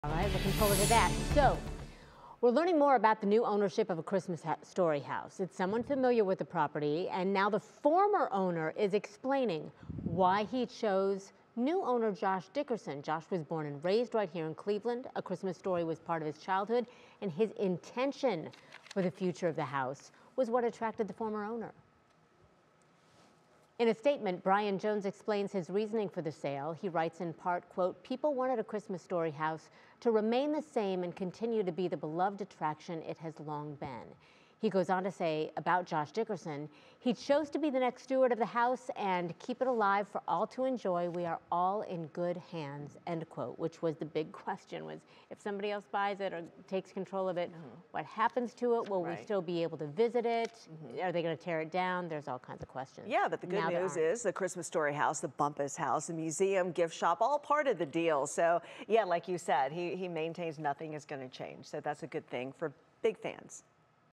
Get to know the new owner of A Christmas Story house in Cleveland
When the iconic Cleveland house used in A Christmas Story first went up for sale last year, it was a moment that captured national attention.

Who would become the new owner? Who was going to take over the legendary property at 3159 West 11th Street in Cleveland's Tremont neighborhood?

While some fans had speculated it might be Ralphie actor Peter Billingsley, the new owner was officially revealed Tuesday morning as Joshua Dickerson.

But who is he?

Dickerson is somebody who has been connected to the house for years -- and now we’re learning even more about how he became the new owner.

Brian Jones, the man who bought the house and transformed it into a bucket list destination years ago, posted the following update on Facebook Wednesday morning to explain more about the decision -- and he also revealed that an actor from the film was among "a couple serious contenders."